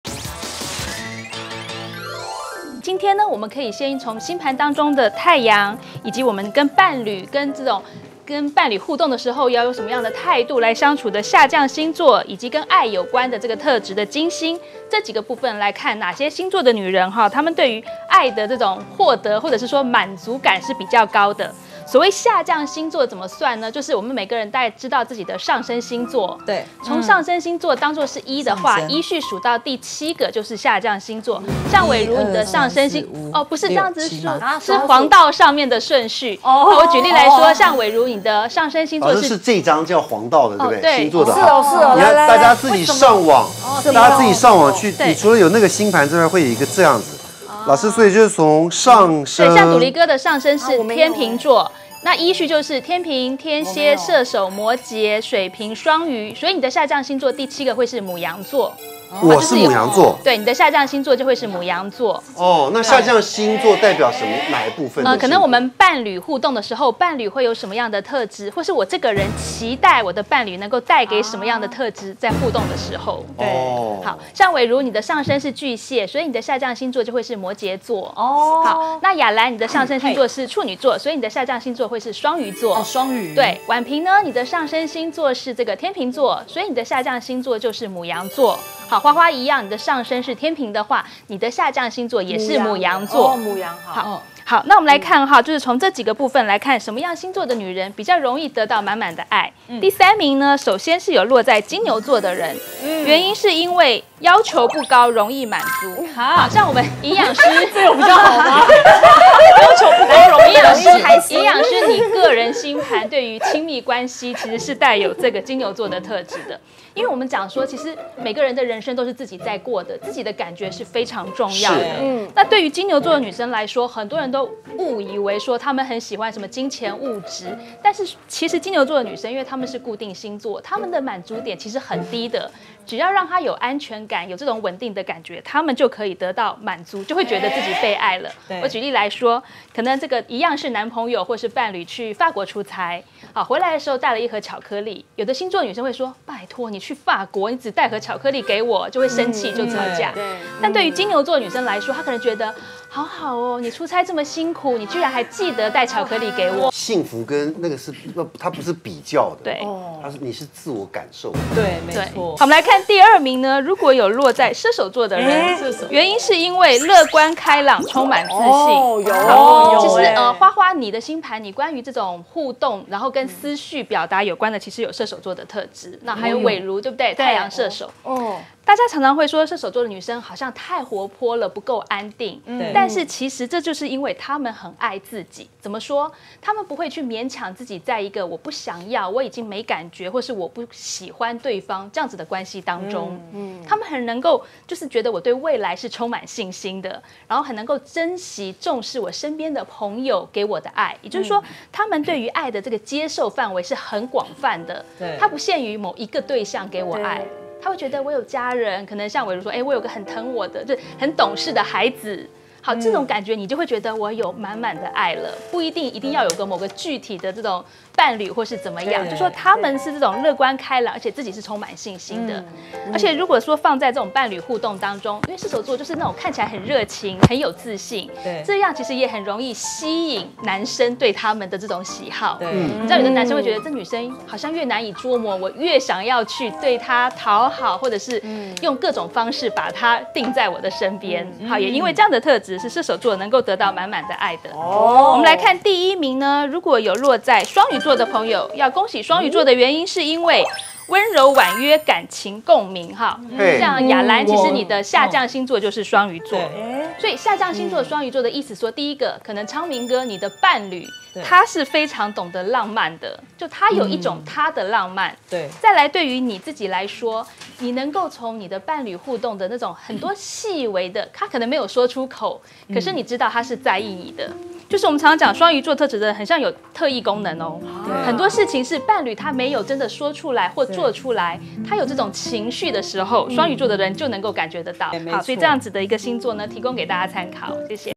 【命運好好玩】天生就是惹人疼 男人最想寵愛的星座女人
有些人天生就是特別有男人緣，快來看看自己有沒有上榜！